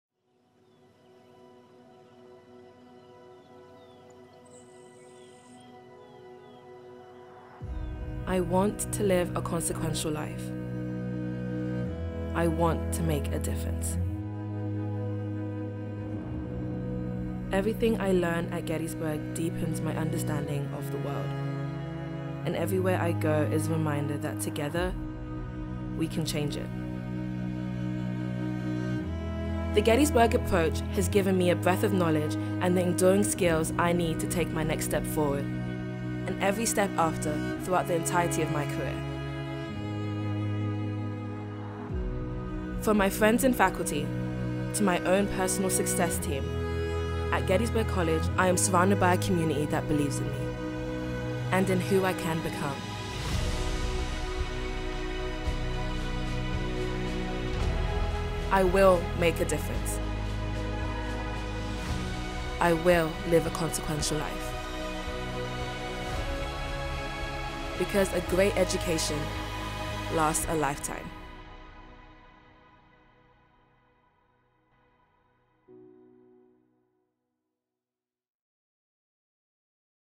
The Gettysburg Approach: Live a consequential life
The Gettysburg Approach is A Consequential Education in practice: a renewed, evolved, and intensive focus on how we deliver our signature undergraduate experience to this generation of Gettysburgians. Designed to benefit the whole student, throughout the entirety of their life and career, the Gettysburg Approach builds within every student a breadth and depth of knowledge and set of enduring skills—cultivated both inside and outside of the classroom—to prepare them to thrive in a world marked by change and adaptation.